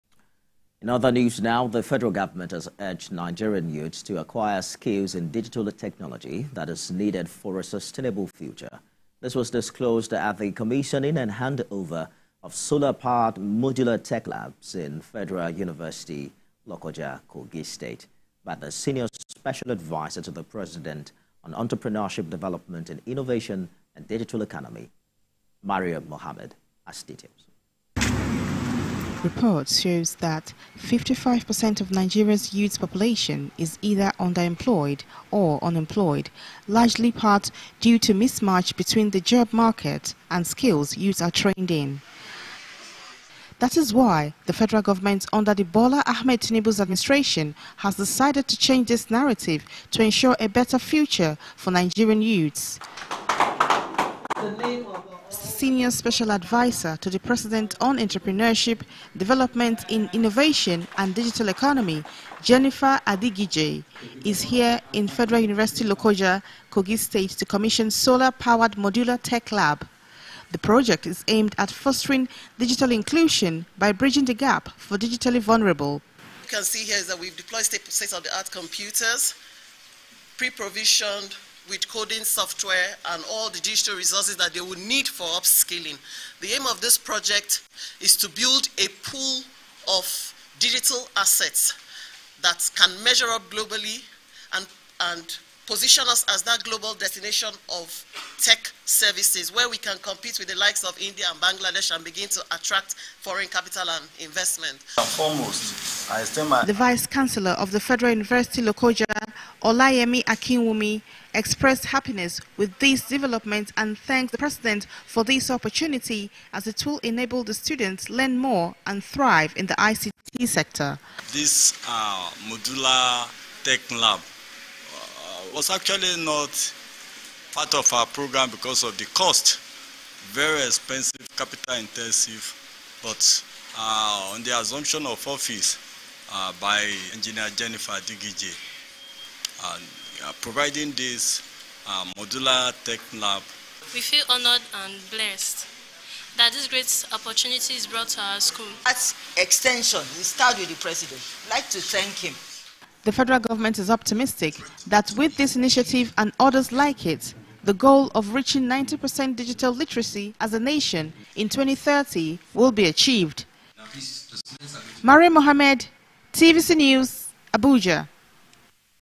Federal University Lokoja Receives Solar-Powered Modular Technology Lab
The Federal Government has urged Nigerian youths to acquire skills in digital technology that is needed for a sustainable future.
This was disclosed at the Commissioning and Handover of solar powered modular tech lab in Federal University Lokoja, Kogi State, by the Senior Special Adviser to the President on Entrepreneurship Development in Innovation and Digital Economy.